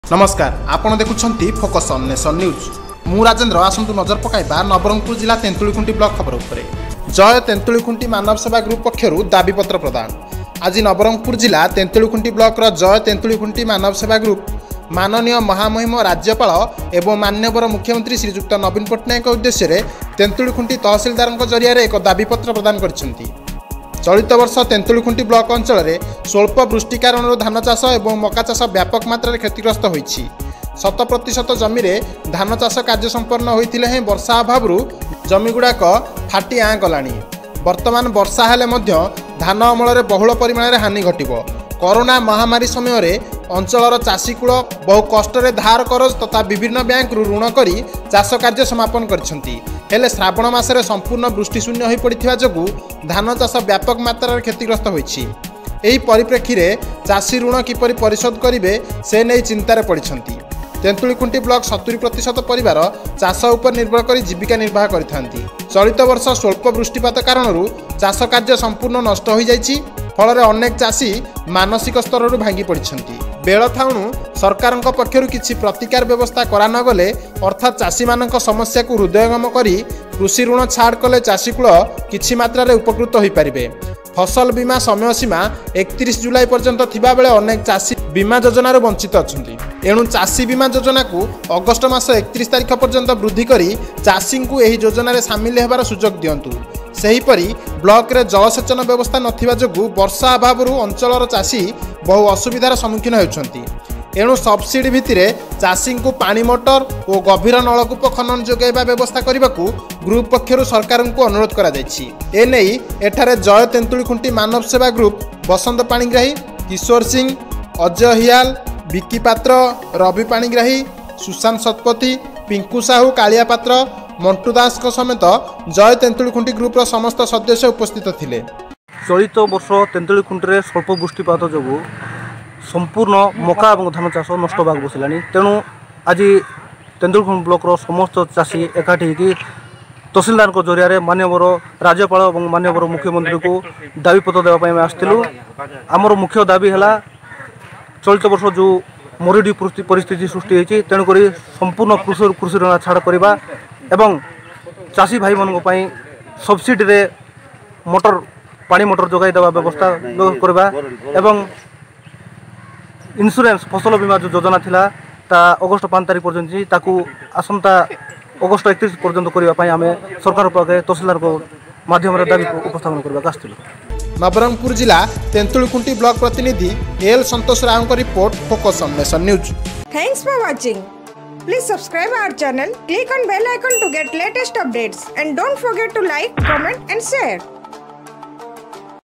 ଜୟ ତେନ୍ତୁଳୀଖୁଣ୍ଟି ମାନବସେବା ଗ୍ରୁପ ପକ୍ଷରୁ ଦାବିପତ୍ର ପ୍ରଦାନ
ଆଜି ନବରଙ୍ଗପୁର ଜିଲ୍ଲା ତେନ୍ତୁଳୀଖୁଣ୍ଟି ବ୍ଲକ ର ଜୟ ତେନ୍ତୁଳୀଖୁଣ୍ଟି ମାନବସେବା ଗ୍ରୁପ ମାନନୀୟ ମହା ମହିମ ରାଜ୍ୟପାଳ ଏବଂ ମାନ୍ୟବର ମୁଖ୍ୟମନ୍ତ୍ରୀ ଶ୍ରୀଯୁକ୍ତ ନବୀନ ପଟ୍ଟନାୟକ ଙ୍କ ଉଦ୍ଧେଶ୍ୟ ରେ ତେନ୍ତୁଳିଖୁଣ୍ଟି ତହସିଲଦାର ଙ୍କ ଜରିଆରେ ଏକ ଦାବିପତ୍ର ପ୍ରଦାନ କରାଯାଇଛି ।
ଜୟ ତେନ୍ତୁଳୀଖୁଣ୍ଟି ଗ୍ରୁପ ଆଡ଼ମୀନ ତଥା  ସମାଜ ସେବୀ ଅଖିଳ ଭତ୍ରା ଙ୍କ ନେତୃତ୍ୱ ରେ ଆଜି ତେନ୍ତୁଳିଖୁଣ୍ଟି ମାନବ ସେବା ଗ୍ରୁପ ଚଳିତ ବର୍ଷ ତେନ୍ତୁଳିଖୁଣ୍ଟି ବ୍ଲକ ଅଂଚଳରେ ସ୍ୱଳ୍ପ ବୃଷ୍ଟି କାରଣରୁ ଧାନ ଚାଷ ଏବଂ ମକା ଚାଷ ବ୍ୟାପକ ମାତ୍ରାରେ କ୍ଷତିଗ୍ରସ୍ତ ହୋଇଛି । ଶତ ପ୍ରତିଶତ ଜମିରେ ଧାନ ଚାଷ କାର୍ଯ୍ୟ ସମ୍ପନ୍ନ ହୋଇଥିଲେ ହେଁ ବର୍ଷା ଅଭାବରୁ ଜମି ଗୁଡାକ ଫାଟି ଆଁ  କଲାଣି । ବର୍ତ୍ତମାନ ବର୍ଷା ହେଲେ ମଧ୍ୟ ଧାନ ଅମଳରେ ବହୁଳ ପରିମାଣରେ ହାନି ଘଟିବ । କରୋନା ମହାମାରୀ ସମୟରେ ଅଂଚଳର ଚାଷୀକୁଳ ବହୁ କଷ୍ଟରେ ଧାର କରଜ ତଥା ବିଭିନ୍ନ ବ୍ୟାଙ୍କ ରୁ ଋଣ କରି ଚାଷ କାର୍ଯ୍ୟ ସମାପନ କରିଛନ୍ତି । ହେଲେ ଶ୍ରାବଣ ମାସରେ ସମ୍ପୂର୍ଣ ବୃଷ୍ଟି ଶୂନ୍ୟ ହୋଇ ପଡ଼ିଥିବା ଯୋଗୁଁ ଧାନ ଚାଷ ବ୍ୟାପକ ମାତ୍ରାରେ କ୍ଷତିଗ୍ରସ୍ତ ହୋଇଛି ! ଏହି ପରିପ୍ରେକ୍ଷୀରେ ଚାଷୀ ଋଣ କିପରି ପରିଶୋଧ କରିବେ ସେନେଇଁ ଚିନ୍ତାରେ ପଡିଛନ୍ତି । ତେନ୍ତୁଳିଖୁଣ୍ଟି ବ୍ଲକର 70 ପ୍ରତିଶତ ପରିବାର, ଚାଷ ଉପରେ ନିର୍ଭର କରି ଜୀବିକା ନିର୍ବାହ କରିଥାନ୍ତି । ଚଳିତ ବର୍ଷ ସ୍ୱଳ୍ପ ବୃଷ୍ଟିପାତ କାରଣରୁ ଚାଷ କାର୍ଯ୍ୟ ସମ୍ପୂର୍ଣ ନଷ୍ଟ ହୋଇଛି, ଫଳରେ  ଅନେକ ଚାଷୀ ମାନସିକ ସ୍ତରରୁ ଭାଙ୍ଗି ପଡିଛନ୍ତି । ବେଳ ଥାଉ ଣୁ ସରକାର ଙ୍କ ପକ୍ଷରୁ କିଛି ପ୍ରତିକାର ବ୍ୟବସ୍ଥା କରାନଗଲେ ଅର୍ଥାତ ଚାଷୀ ମାନଙ୍କ ସମସ୍ୟା କୁ ହୃଦୟଙ୍ଗମ କରି କୃଷି ଋଣ ଛାଡ଼ କଲେ ଚାଷୀକୁଳ କିଛି ମାତ୍ରାରେ ଉପକୃତ ହୋଇପାରିବେ ।  ଫସଲ ବୀମା ସମୟ ସୀମା 31 ଜୁଲାଇ ପର୍ଯ୍ୟନ୍ତ ଥିବାବେଳେ ଅନେକ ଚାଷୀ ବୀମା ଯୋଜନାରୁ ବଂଚିତ ଅଛନ୍ତି । ଏଣୁ ଚାଷୀ  ବୀମା ଯୋଜନା କୁ ଅଗଷ୍ଟ ମାସ 31 ତାରିଖ ପର୍ଯ୍ୟନ୍ତ ବୃଦ୍ଧି କରି ଚାଷୀଙ୍କୁ ଏହି ଯୋଜନାରେ ସାମିଲ ହେବାର ସୁଯୋଗ ଦିଅନ୍ତୁ । ସେହିପରି ବ୍ଲକରେ ଜଳ ସେଚନ ବ୍ୟବସ୍ଥା ନଥିବା ଯୋଗୁଁ ବର୍ଷା ଅଭାବରୁ ଅଂଚଳର ଚାଷୀ ବହୁ ଅସୁବିଧାର ସମ୍ମୁଖୀନ ହେଉଛନ୍ତି । ଏଣୁ ସବସିଡି ଭିତିରେ ଚାଷୀଙ୍କୁ ପାଣି ମଟର ଓ  ଗଭୀର ନଳକୂପ ଖନନ  ଯୋଗେଇବା ବ୍ୟବସ୍ଥା କରିବାକୁ ଗ୍ରୁପ ପକ୍ଷରୁ ସରକାର ଙ୍କୁ  ଅନୁରୋଧ କରାଯାଇଛି ।
ଏନେଇ ଏଠାରେ "ଜୟ ତେନ୍ତଳିଖୁଣ୍ଟି ମାନବ ସେବା ଗ୍ରୁପ୍"ବସନ୍ତ ପାଣିଗ୍ରାହୀ, କିଶୋର ସିଂ, ଅଜୟ ହିଆଲ, ବିକି ପାତ୍ର, ରବି ପାଣିଗ୍ରାହୀ, ଶୁଶାନ୍ତ ଶତପଥି, ପିଙ୍କୁ ସାହୁ, କାଳିଆ ପାତ୍ର, ମଣ୍ଟୁ ଦାସ ଙ୍କ ସମେତ ଜୟ ତେନ୍ତୁଳୀଖୁଣ୍ଟି ଗ୍ରୁପ ର ସମସ୍ତ ସଦସ୍ୟ ଉପସ୍ଥିତ ଥିଲେ ।